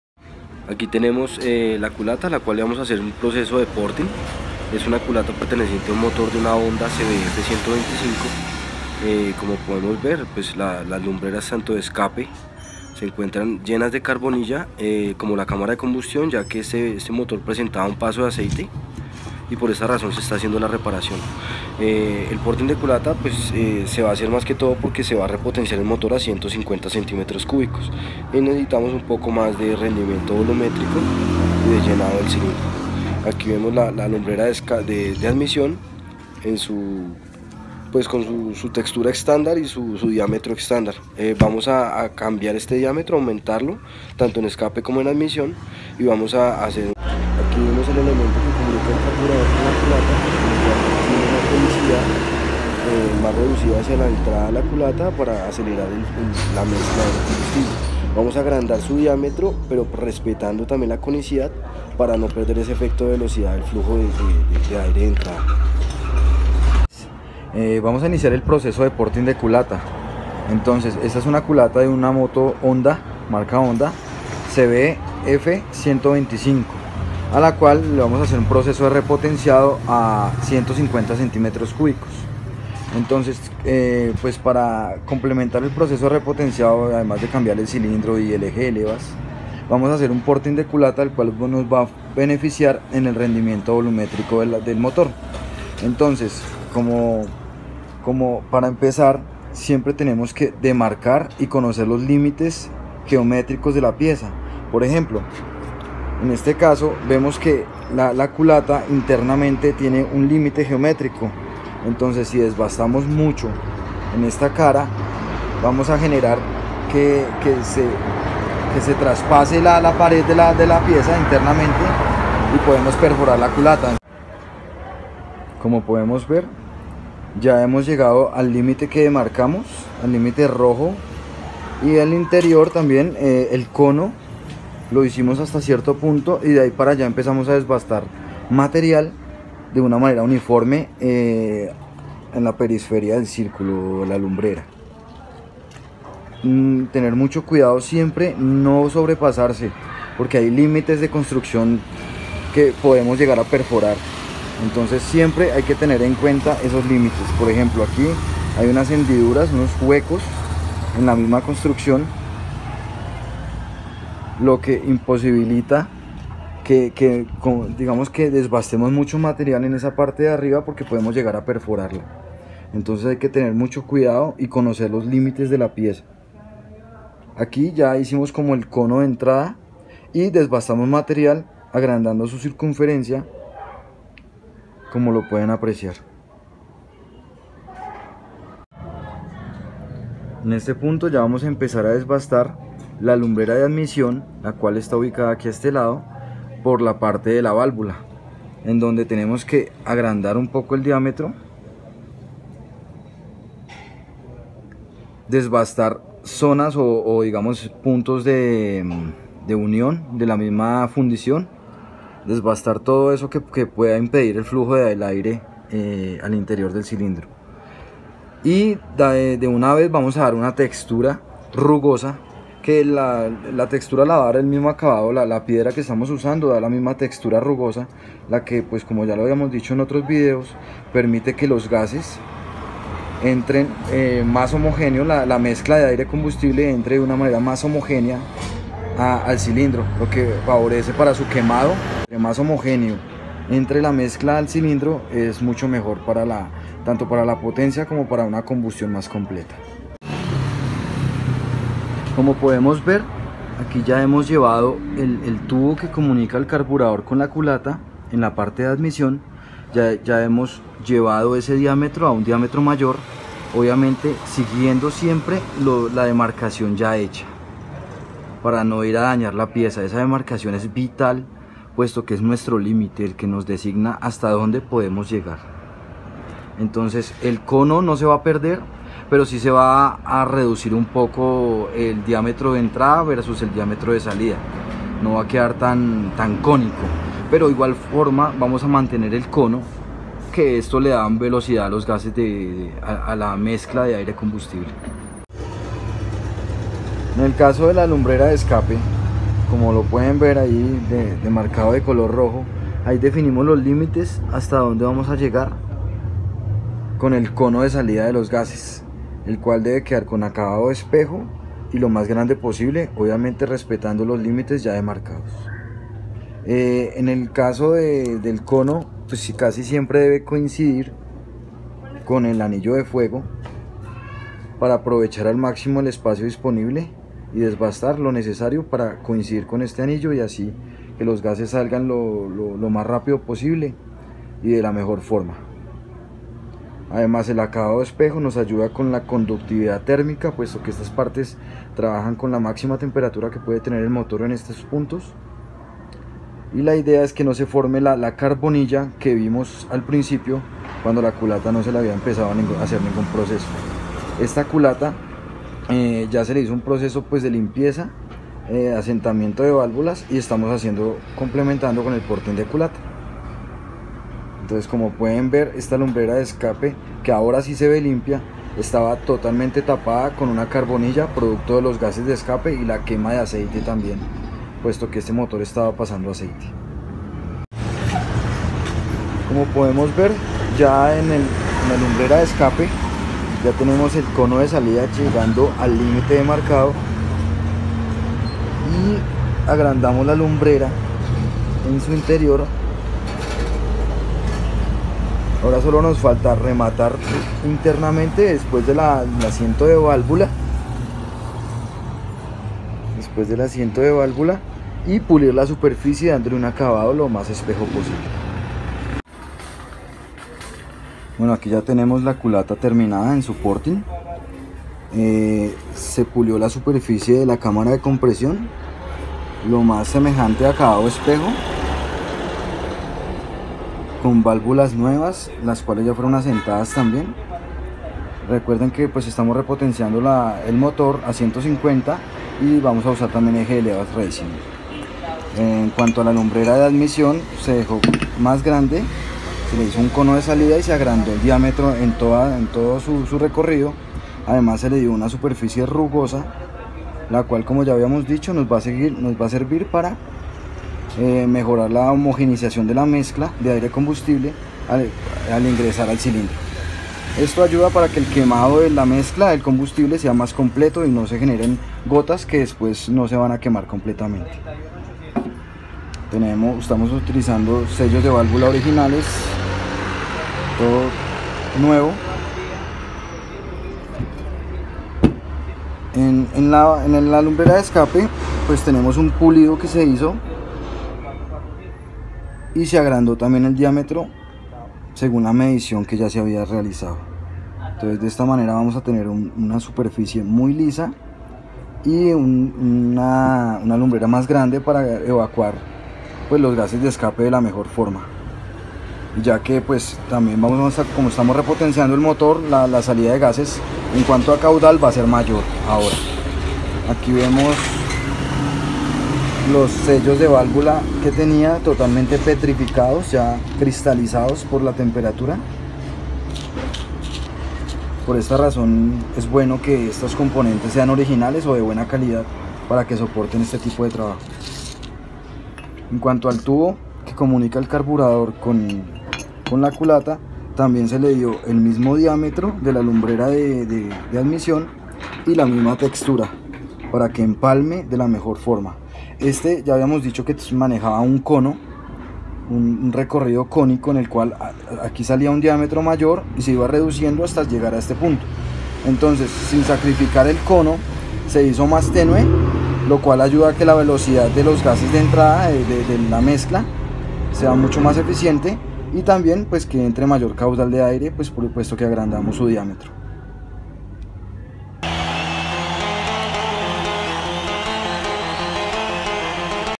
Porting de culata, honda CBF125
Proceso de porting de para motor de motocicleta de la marca honda de referencia cbf125, la cual se repotencio a 150cc.

Bogota - Colombia

FC Garage
Fabián cruz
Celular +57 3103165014
Facebook  FC Garage